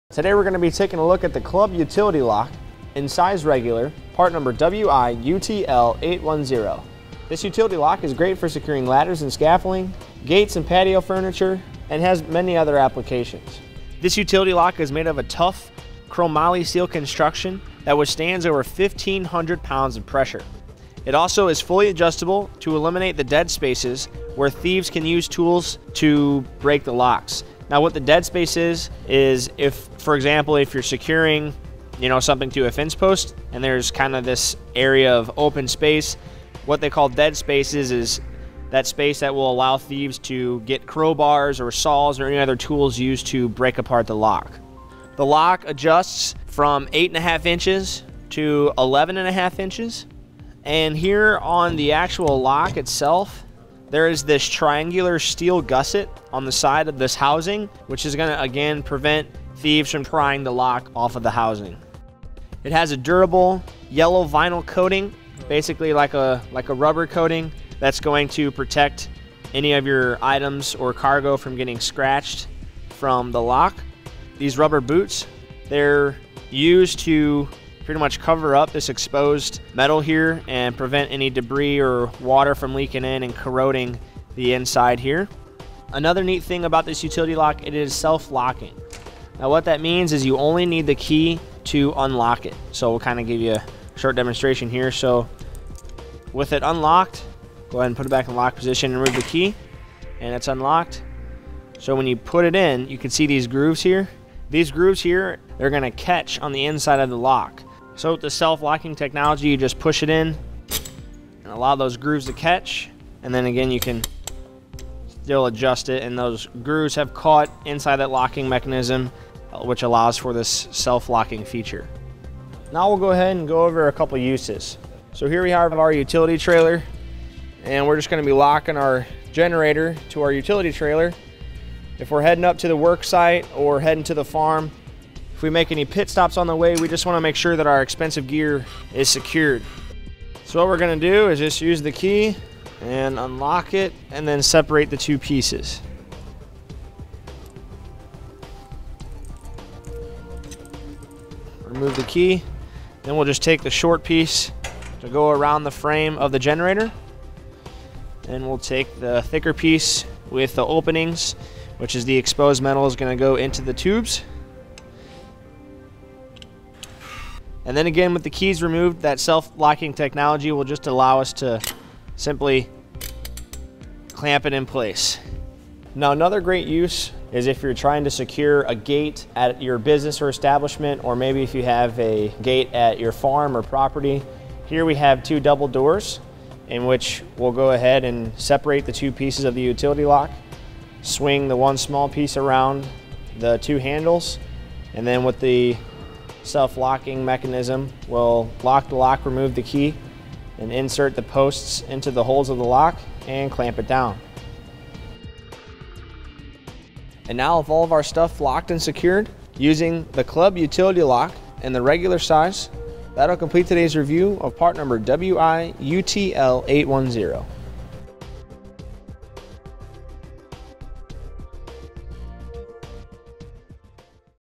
etrailer | A Closer Look at the The Club Regular Utility Lock
Today were going to be taking a look at the Club Utility Lock in size regular. Part number WIUTL810. This utility lock is great for securing ladders and scaffolding, gates and patio furniture, and has many other applications. This utility lock is made of a tough chrome alloy steel construction that withstands over 1,500 pounds of pressure. It also is fully adjustable to eliminate the dead spaces where thieves can use tools to break the locks. What the dead space is, is if, for example, if youre securing something to a fence post and there is kind of this area of open space. What they call dead space, is that space that will allow thieves to get crowbars or saws, or any other tools used to break apart the lock. The lock adjusts from 8  inches to 11  inches. Here on the actual lock itself, there is this triangular steel gusset on the side of this housing, which is going to, again, prevent thieves from prying the lock off of the housing. It has a durable, yellow vinyl coating.

Basically, like a rubber coating thats going to protect any of your items or cargo from getting scratched from the lock. These rubber boots, theyre used to pretty much cover-up this exposed metal here, and prevent any debris or water from leaking in and corroding the inside here. Another neat thing about this utility lock, it is self-locking. What that means is you only need the key to unlock it. Well kind of give you a short demonstration here. With it unlocked, go ahead and put it back in locked position and remove the key, and its unlocked. When you put it in, you can see these grooves here.

These grooves here are going to catch on the inside of the lock. With the self-locking technology, you just push it in and allow those grooves to catch, and then, again, you can still adjust it. Those grooves have caught inside that locking mechanism, which allows for this self-locking feature. Now well go ahead and go over a couple uses. Here we have our utility trailer and were just going to be locking our generator to our utility trailer. If were heading up to the work site, or heading to the farm, if we make any pit stops on the way, we just want to make sure that our expensive gear is secured. What were going to do is just use the key and unlock it, and then separate the two pieces. Remove the key, and then well just take the short piece to go around the frame of the generator.

Then well take the thicker piece with the openings, which the exposed metal is going to go into the tubes. Then, again with the keys removed, that self-locking technology will just allow us to simply clamp it in place. Another great use is if youre trying to secure a gate at your business or establishment, or maybe if you have a gate at your farm or property. Here we have two double doors, in which well go ahead and separate the two pieces of the utility lock. Swing the one small piece around the two handles, and then with the self-locking mechanism, well lock the lock, remove the key, and insert the posts into the holes of the lock, and clamp it down. Now with all of our stuff locked and secured using the Club Utility lock, in the regular size, that will complete todays review of part number WIUTL810. .